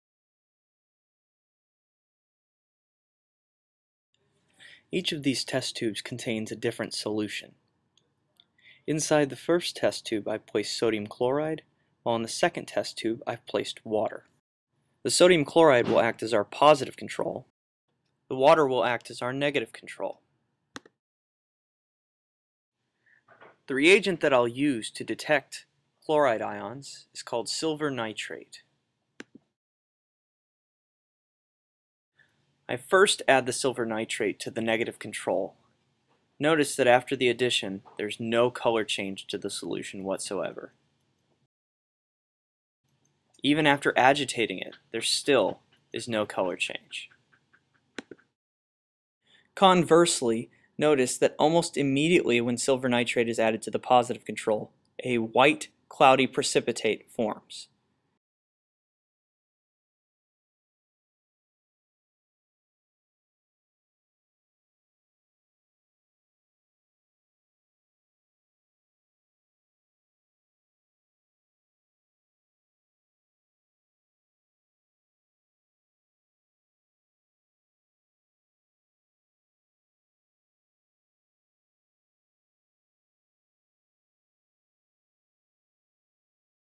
Lab Protocol - Chloride Ion Detection (Unit 7 Diffusion)
In this video I demonstrate the procedure for detecting the presence of chloride ions in a soluiton